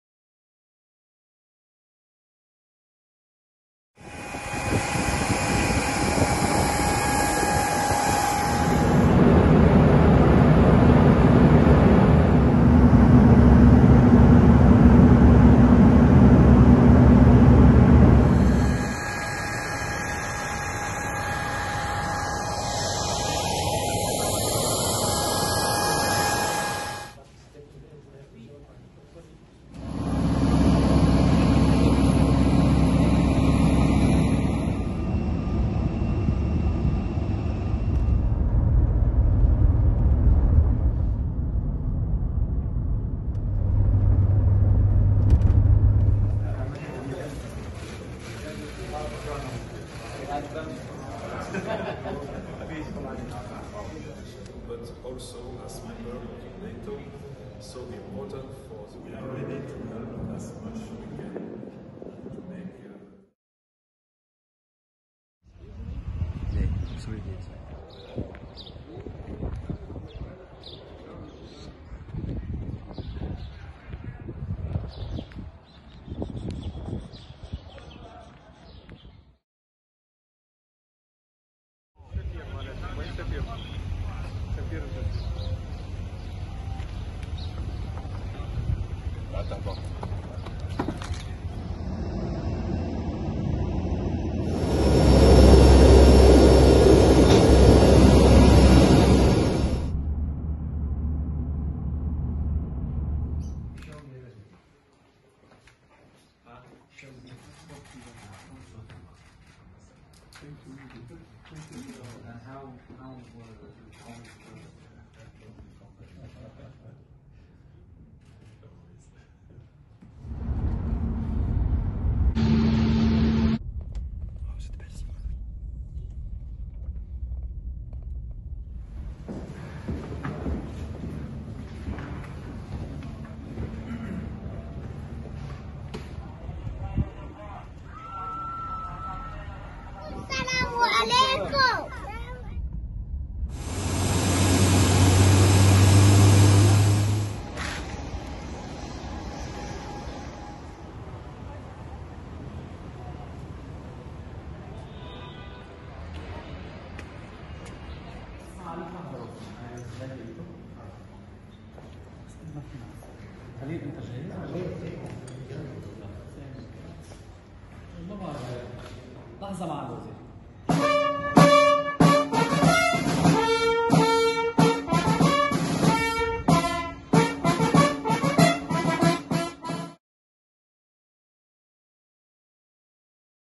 Toutes les rencontres de Philippe Goffin au Moyen-Orient lors de sa tournée diplomatique
Dans le contexte de la présidence belge au Conseil de sécurité des Nations-Unies, la mission de Philippe Goffin au Moyen-Orient s’inscrit dans une volonté de comprendre les enjeux sécuritaires et de paix dans la région.